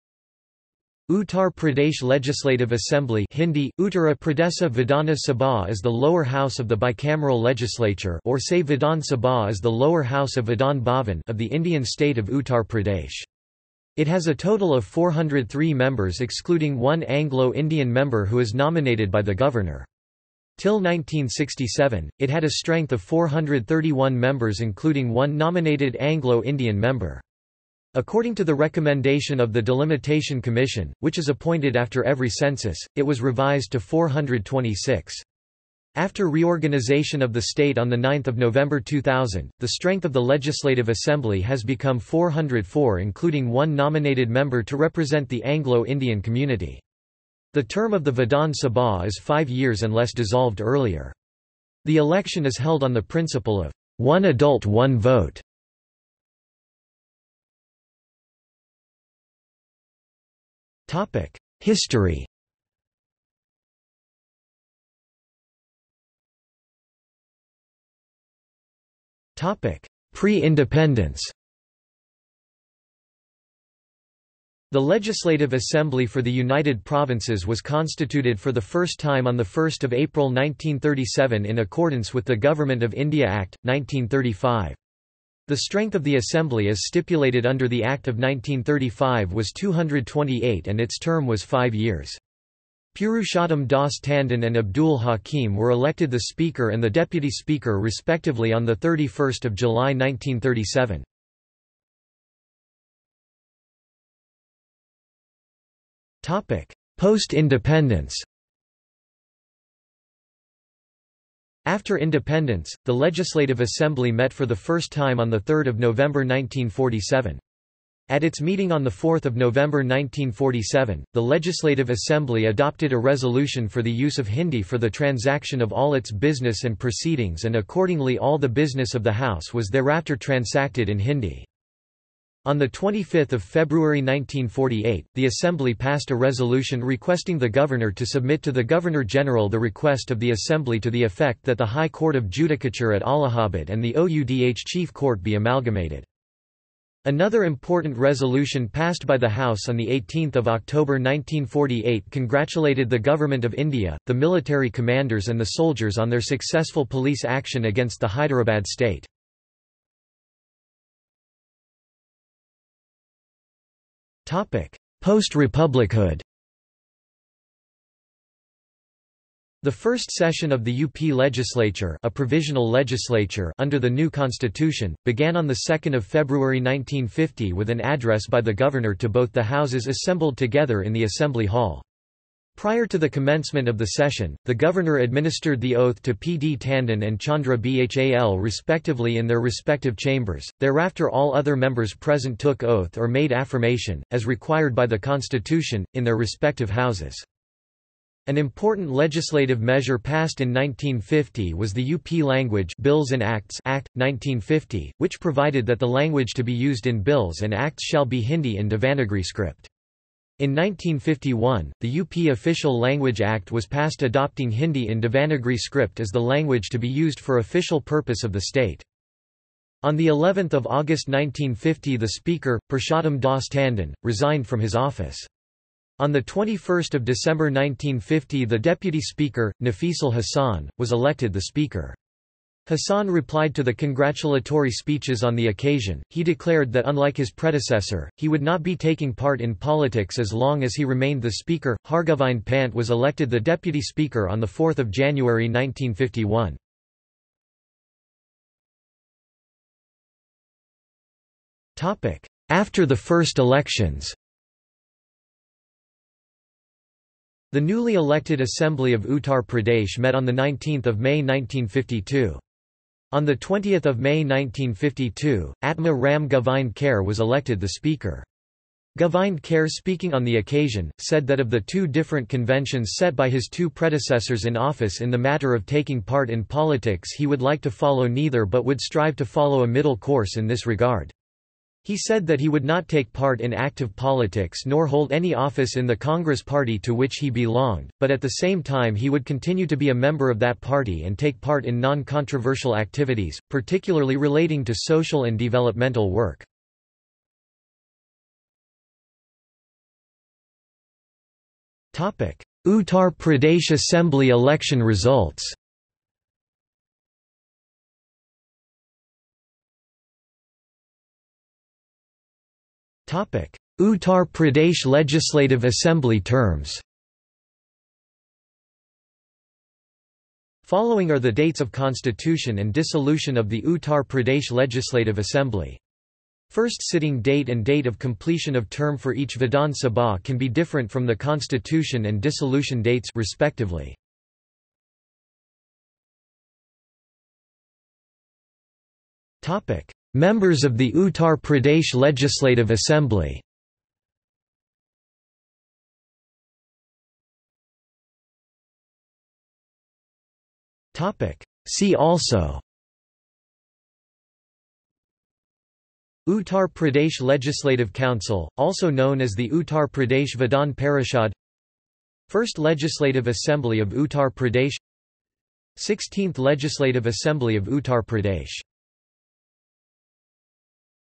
Uttar Pradesh Legislative Assembly | Wikipedia audio article
This is an audio version of the Wikipedia Article:
Uttar Pradesh Legislative Assembly

00:00:57 1 History
00:01:06 1.1 Pre-independence
00:01:42 1.2 Post-independence
00:02:40 1.3 Post-republichood
00:04:08 1.4 After the first elections
00:04:58 2 Uttar Pradesh Assembly Election Results
00:05:09 3 Uttar Pradesh Legislative Assembly terms
00:05:35 4 Members of the Uttar Pradesh Legislative Assembly
00:05:47 5 See also

- Socrates

SUMMARY
Uttar Pradesh Legislative Assembly (Hindi: उत्तर प्रदेश विधान सभा) is the lower house of the bicameral legislature (or say Vidhan Sabha is the lower house of Vidhan Bhavan) of the Indian state of Uttar Pradesh. It has a total of 403 members excluding one Anglo-Indian member who is nominated by the Governor. Till 1967, it had a strength of 431 members including one nominated Anglo-Indian member. According to the recommendation of the Delimitation Commission, which is appointed after every Census, it was revised to 426. After reorganization of the State on 9 November 2000, the strength of the Legislative Assembly has become 404 including one nominated member to represent the Anglo-Indian community. The Term of the Vidhan Sabha is five years unless dissolved earlier. The election is held on the principle of "one adult one vote".